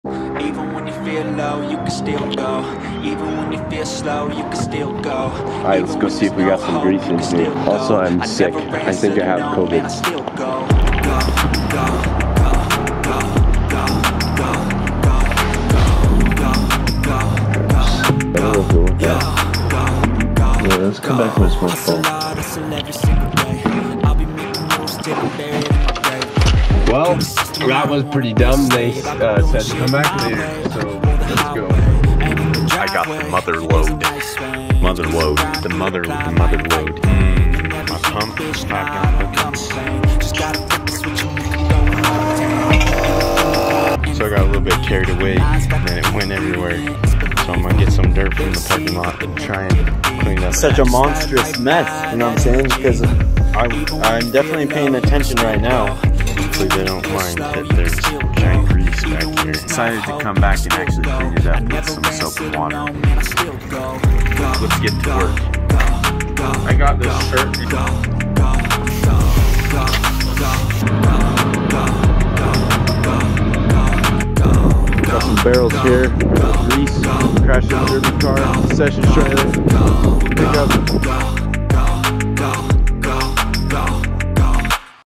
Collecting Grease (I had to clean up a mess too)
Started to collect some grease for the grease wagon. My goal is to convert my diesel pickup to run on used veggie oil because it's environmentally friendly, it's cheap, and it's freakin cool. My goal was to drive to Alaska for the summer on used veggie oil but because it's taking so long, and the summer is quickly disappearing, I might just have to do the veggie conversion when I get back.

I've been waiting for the kit I ordered to put on my pickup, as well as the centrifuge for over two months now but I thought I would get a head start on collecting the oil.

keep it greasy out there